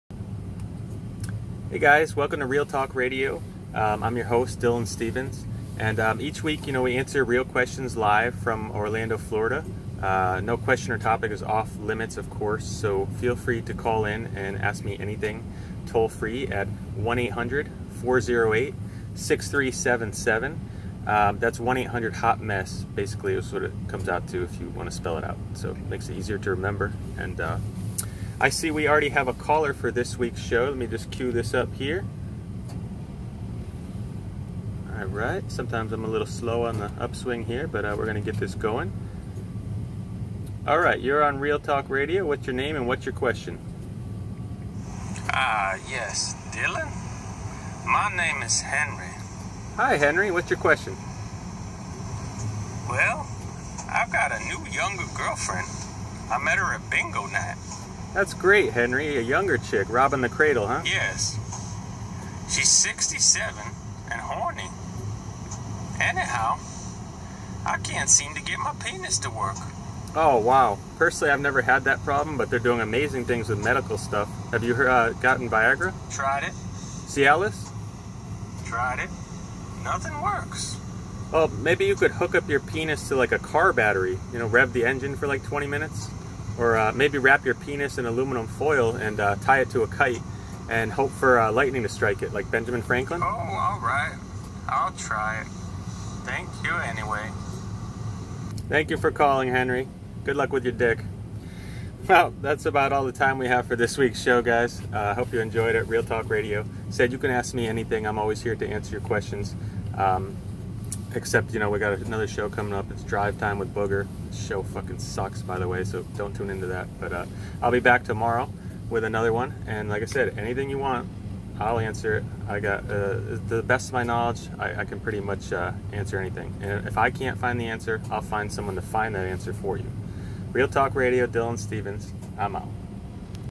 Real Talk Radio Podcasts Episode 3 Erectile Dysfunction
podcasts about relationships and podcasts about love in this funny videos try not to laugh live radio show from orlando florida this episode is about viagra and cialis not helping erectile dysfunction part of a podcast playlist